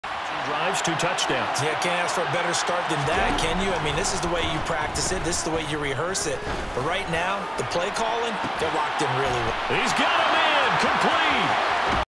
DJ CHARK OH MY…
Bryce Young on the Panthers franchise #36
DJ Chark made this unbelievable catch!